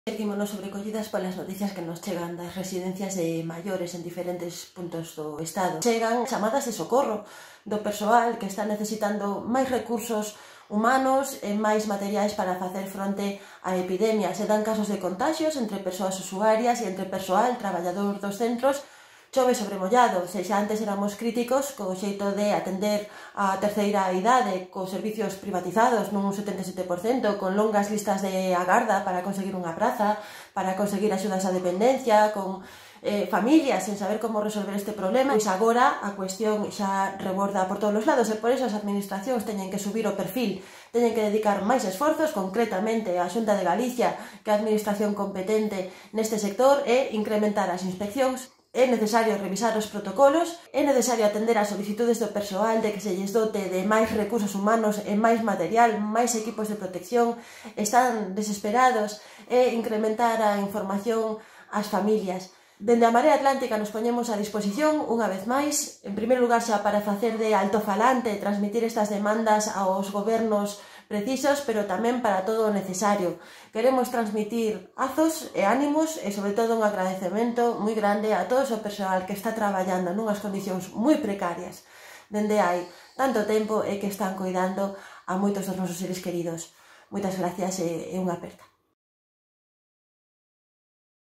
Inspeccións detalladas nas residencias de maiores.
María García reclama máis inspeccións e material de protección nas residencias de maiores durante esta crise sanitaria.

O 77% das que funcionan en Galicia son privadas, máis que en ningunha outra comunidade do Estado.

Coidan de poboación de risco, pero o seu funcionamento é opaco. Hai que supervisalas e actuar para que non se repitan as terribles noticias que chegan estes días.